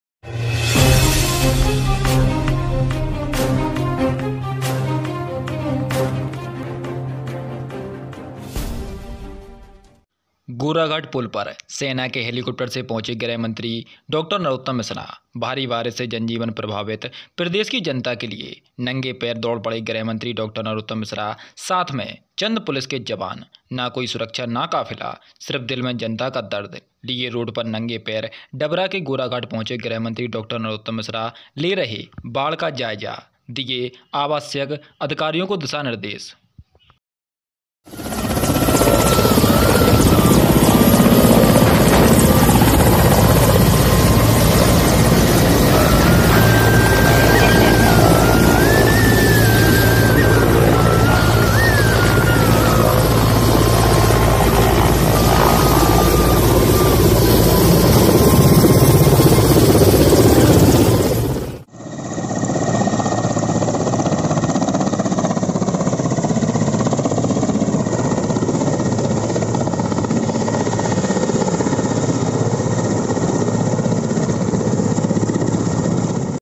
डबरा, गोराघाट पुल पर सेना के हेलीकॉप्टर से नंगे पैर पहुचे गृह मंत्री डॉ नरोत्तम मिश्रा
गोराघाट पुल पर सेना के हेलीकॉप्टर से पहुचे गृह मंत्री डॉ नरोत्तम मिश्रा

भारी बारिश से जनजीवन प्रभावित , प्रदेश की जनता के लिए नंगे पैर दौड़ पड़े गृह मंत्री डॉ नरोत्तम मिश्रा, साथ से चंद पुलिस के जवान, आज न कोई सुरक्षा न काफिला सिर्फ दिल मे जनता का दर्द लिए रोड पर नंगे पैर डबरा के गोराघाट पहुँचे गृह मंत्री डॉ नरोत्तम मिश्रा